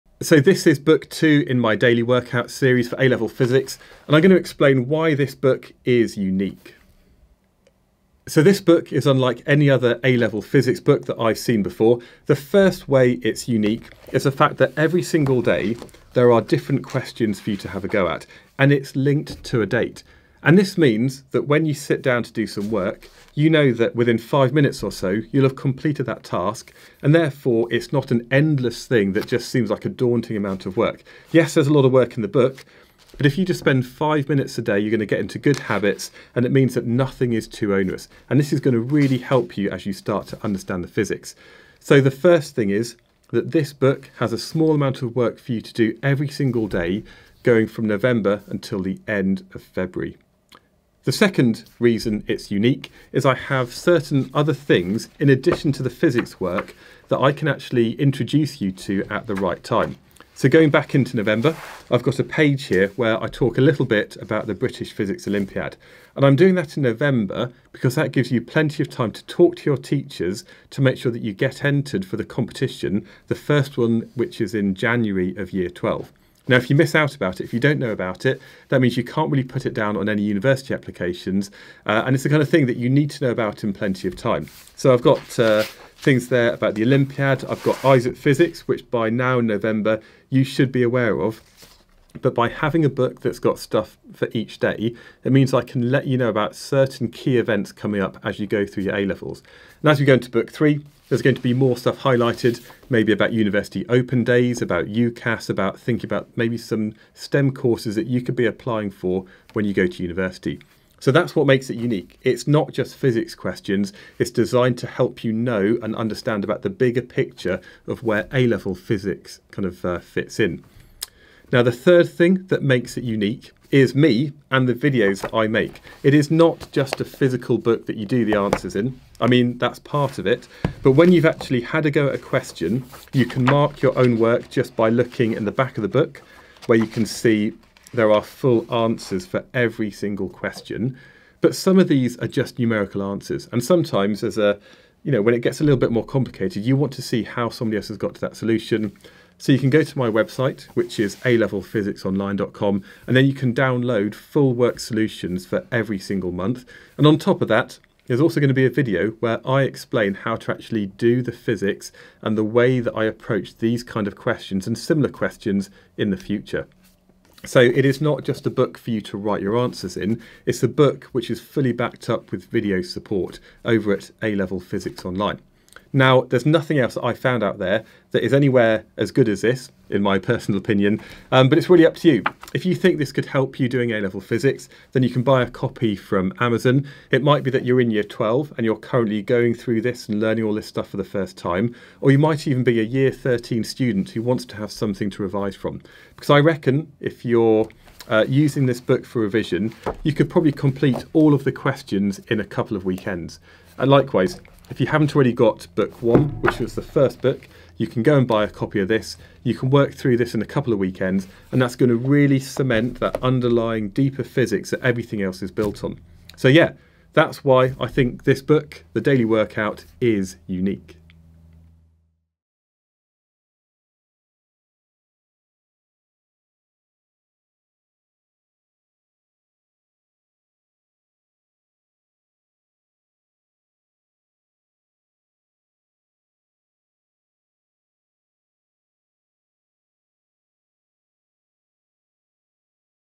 3 THINGS THAT MAKE THIS BOOK UNIQUE
This is Book 2 from my Daily Workout series and why I think it is unique.